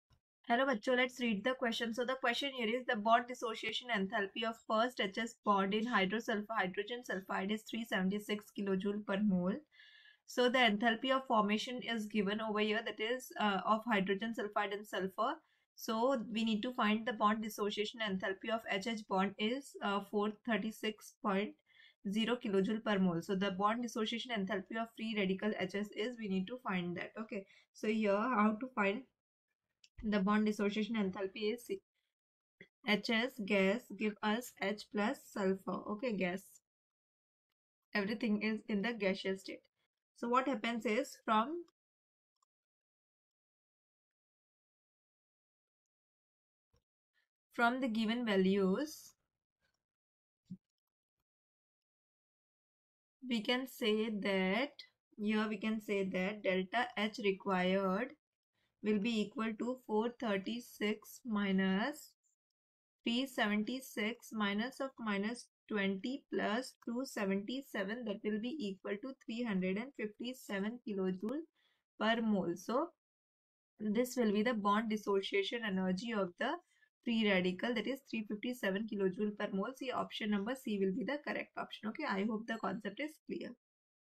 Comprehension X The bond dissociation enthalpy of the first H_-S bond in hydrogen sulphide is 376...
Comprehension X
The bond dissociation enthalpy of the first H_-S bond in hydrogen sulphide is 376.0  kJ / mol. The enthalpies of formation of H_2 S( g) and S(g) are -20.0 and 277.0  kJ / mol, respectively. The bond dissociation enthalpy of H-H bond is 436.0  kJ / mol.
The bond dissociation enthalpy of the free radical HS is
(a) 138  kJ / niol
(b) 276  kJ / mol
(d) 376  kJ / mol